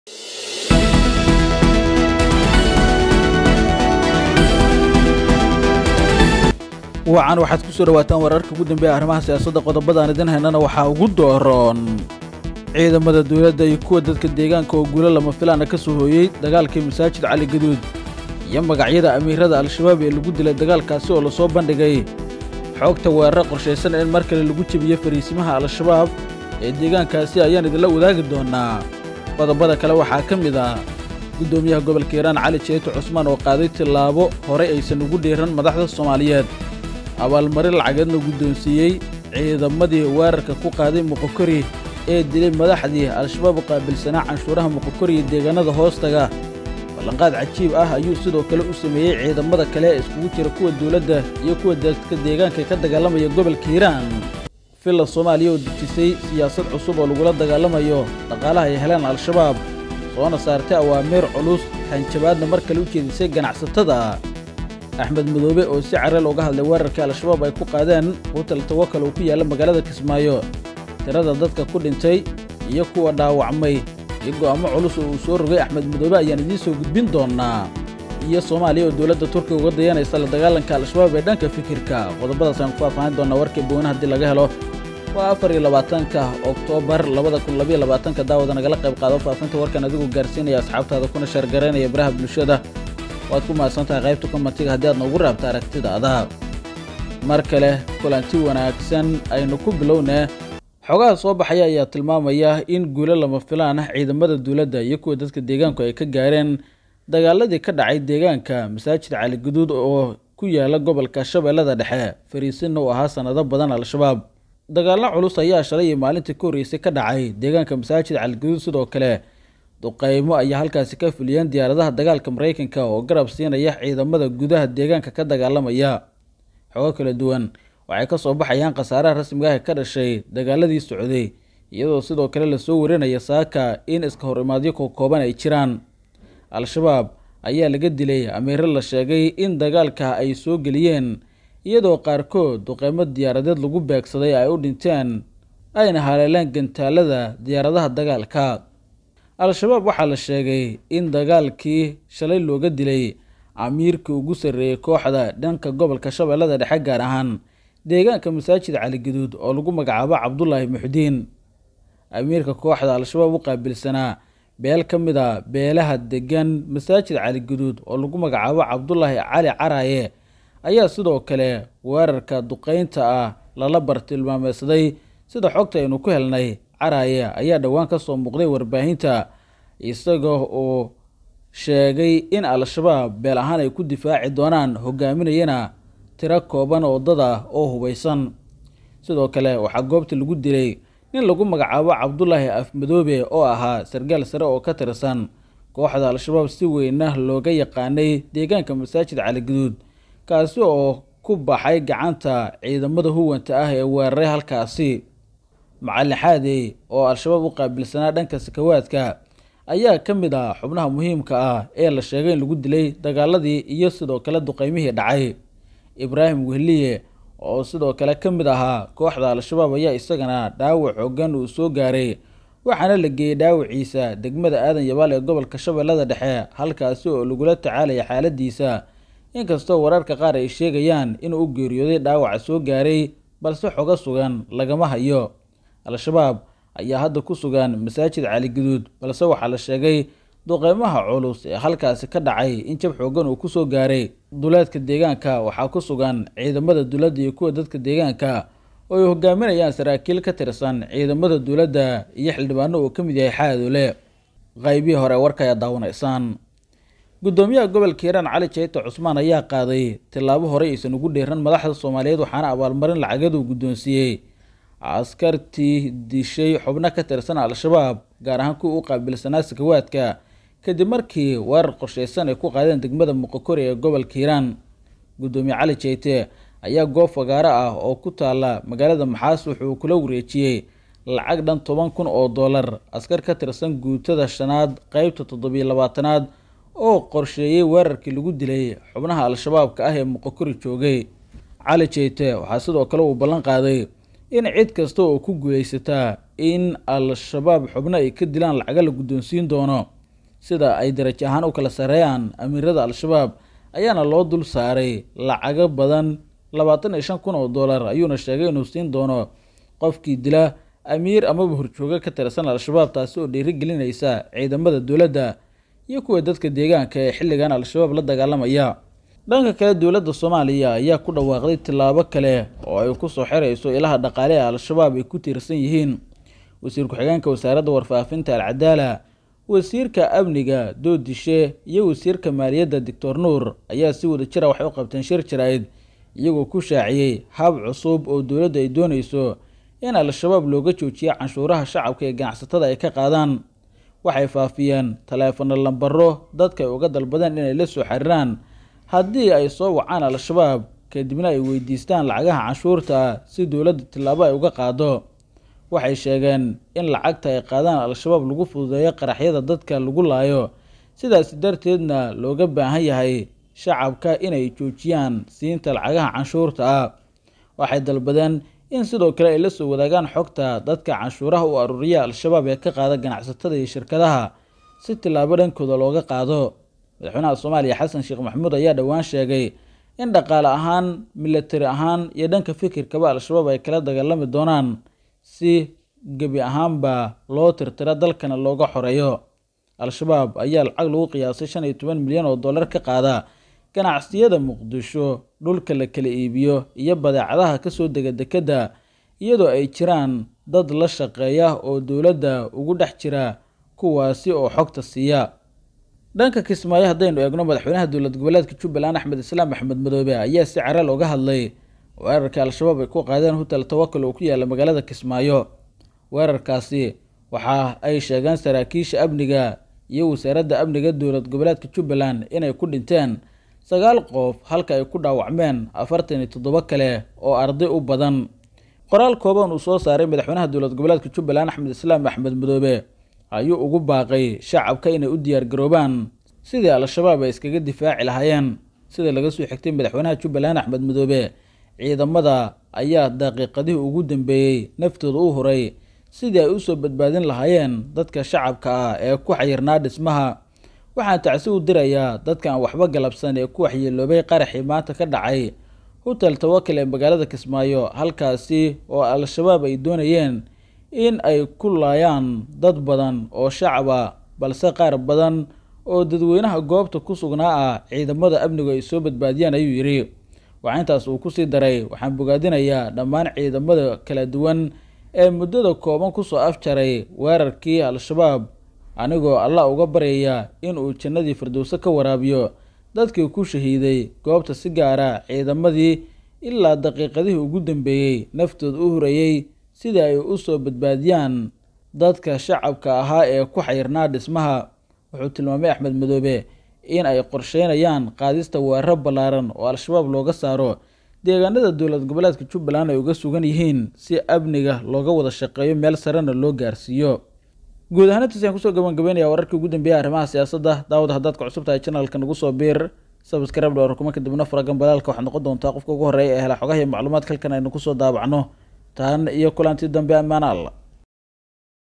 Deg Deg: Magacyada Horjoogayaal sare oo Shabaab laga dilay & MD Xasan Sh oo soo saaray Go'aankii..
Saacadle24 | Xogaha Qarsoon | Wararka Somaaliya |